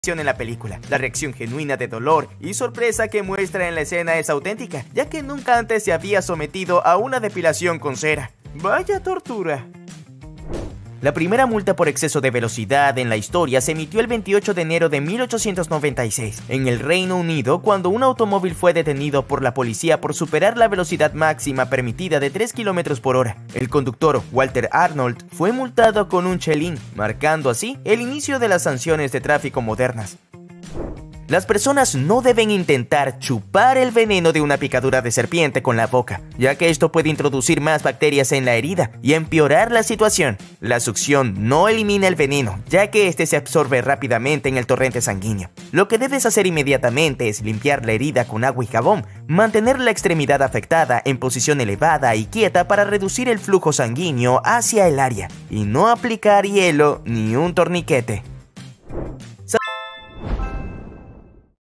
La reacción genuina de dolor y sorpresa en la depilación con cera
La reacción genuina de dolor y sorpresa en la escena de la película es auténtica, ya que el actor nunca antes se había depilado con cera. La primera multa por exceso de velocidad se emitió en 1896 en el Reino Unido, por superar solo 3 km/h. Esto marcó el inicio de las sanciones de tráfico modernas. Además, se menciona que las personas no deben intentar chupar el veneno. Creditos Curi/UnPocodeTodo